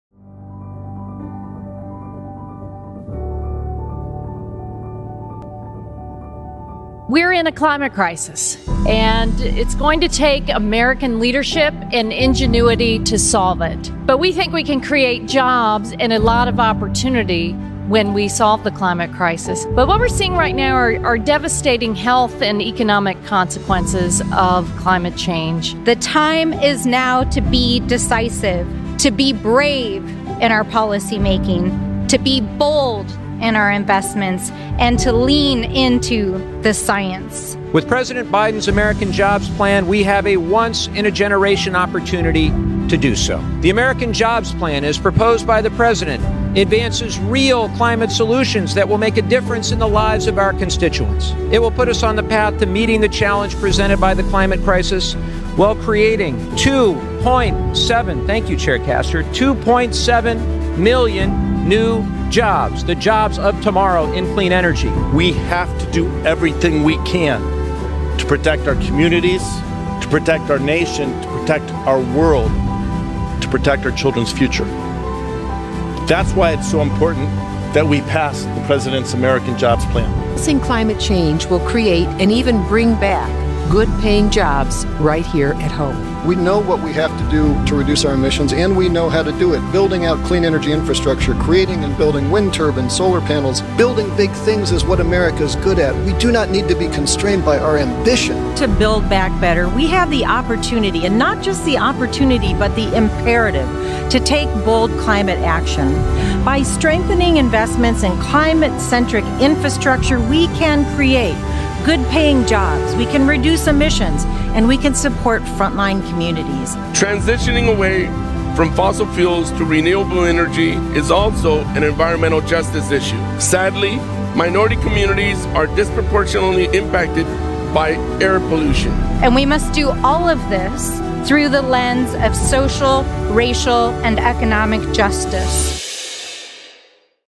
Congressional Champs Demand Climate Action In Recovery Package
After running on the most ambitious climate platform of any presidential nominee, President Biden has proposed a bold infrastructure and recovery package: the American Jobs Plan (AJP). However, the bipartisan package being developed does not include all the critical pieces of the AJP, nor does it contain the required scale of  climate action.

Thankfully, a group of climate champions -- led by Select Committee on the Climate Crisis Chair Kathy Castor -- went to the House floor to reiterate that demand and lay out why they won’t support a recovery package that fails to include sufficiently strong climate provisions and investments. This set of champions -- including Representatives Suzanne Bonamici,  Salud Carbajal, Sean Casten, Marcy Kaptur, Mike Levin, Brad Schneider,and Melanie Stansbury -- made it clear that a package without investments in clean energy, climate resiliency, or provisions to ensure clean air and clean water would have no path forward in the House, either.